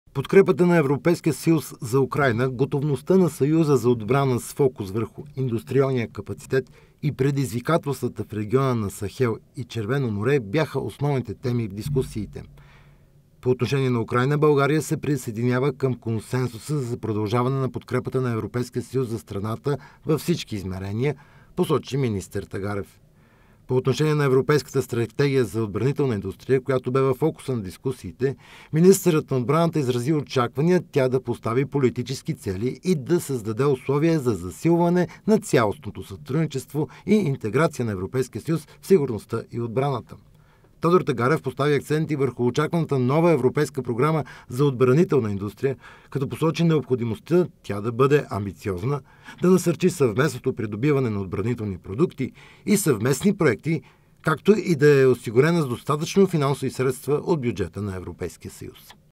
Военните министри на ЕС обсъждат европейската стратегия за отбранителна индустрия
Европейската стратегия за отбранителна индустрия следва да очертае ефективни мерки за повишаване на производството, насърчаване на иновациите и конкурентоспособността в отбранителния сектор. Това заяви министърът на отбраната Тодор Тагерев в Брюксел, където участва на неформална среща на военните министри на страните членки от ЕС.